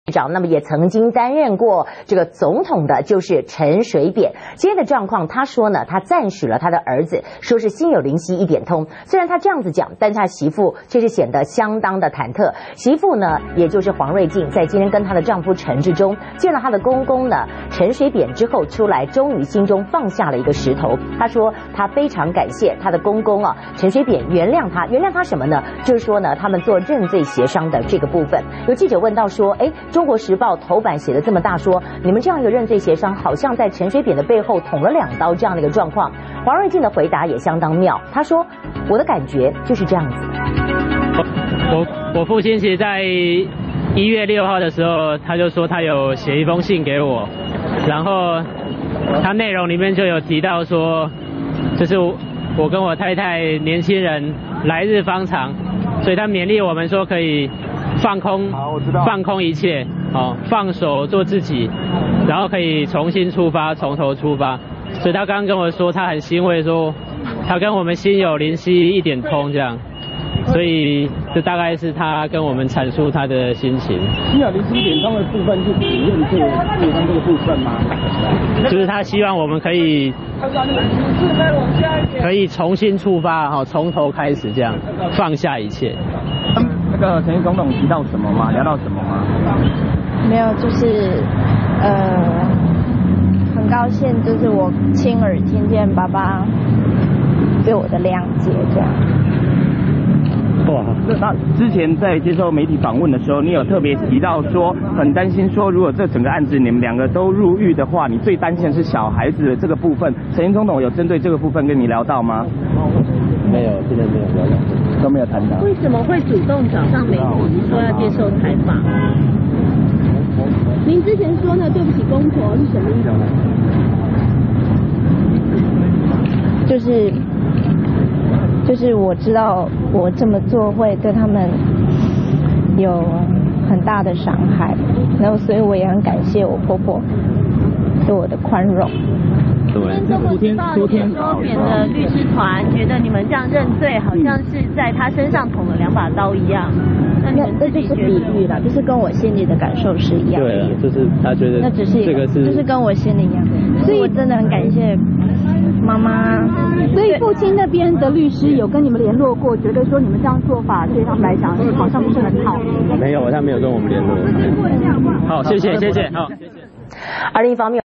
致靚認罪 扁讚許「心有靈犀一點通」
陳水扁讚許兒子心有靈犀一點通，但是他的媳婦黃睿靚卻顯得十分忐忑。